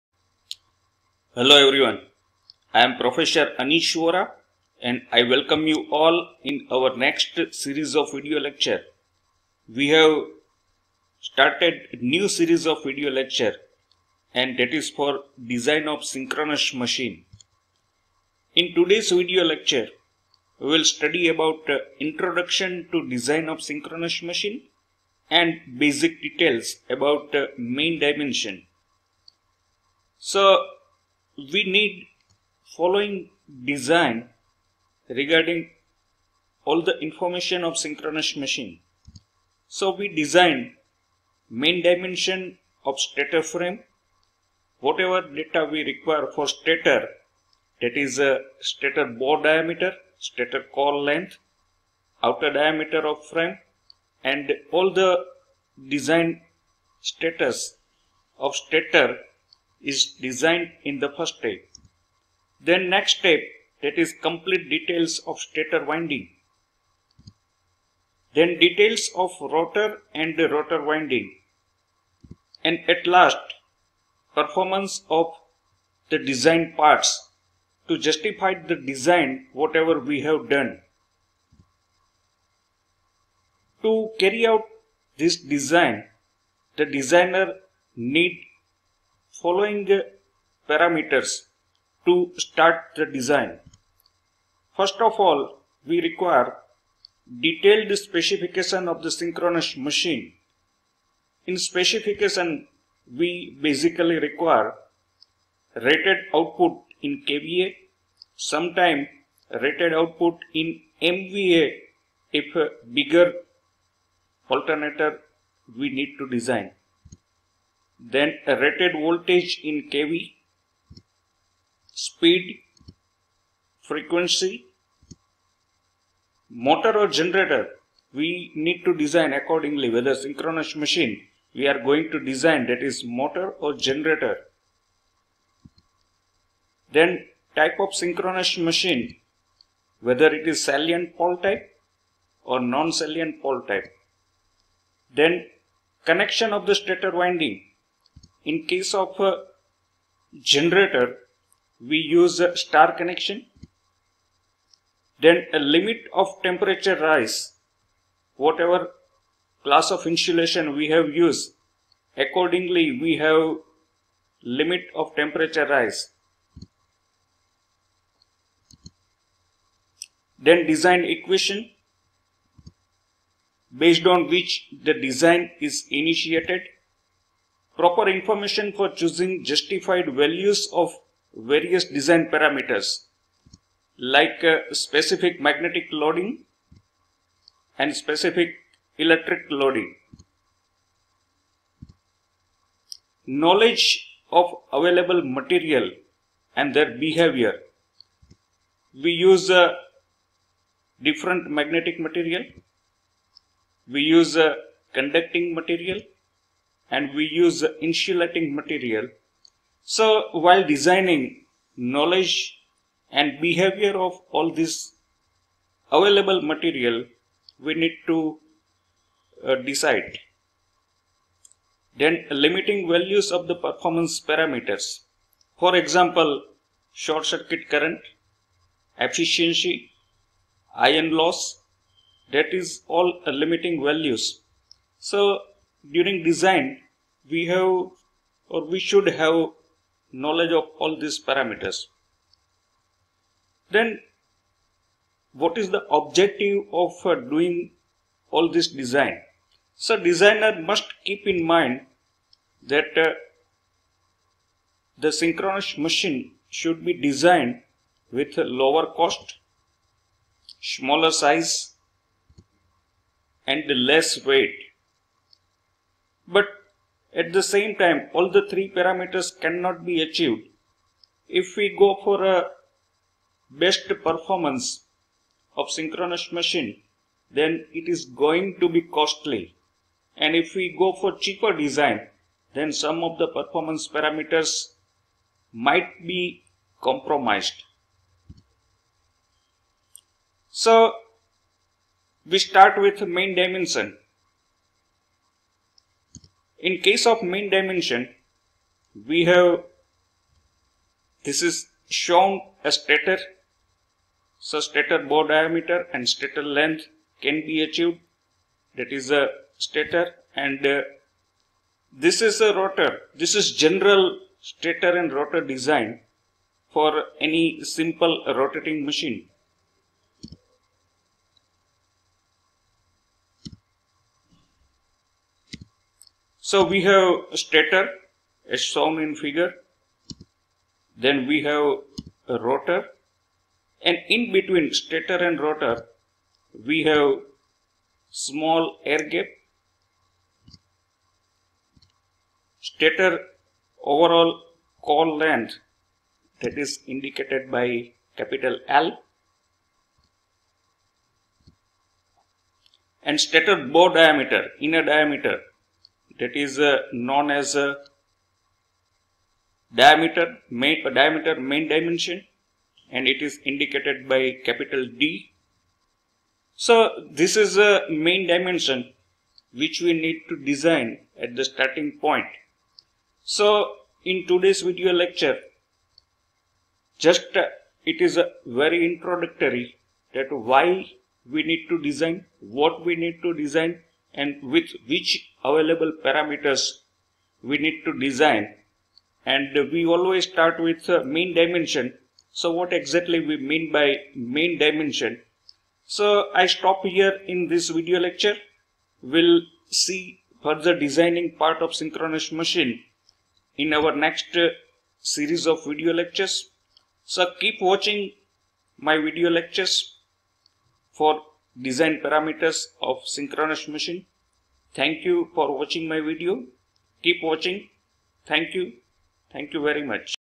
Introduction to Design of Synchronous Machine & Main Dimension
In this video, introductory  design steps for synchronous machine is explained.
Specification
Main Dimension
Objective of Design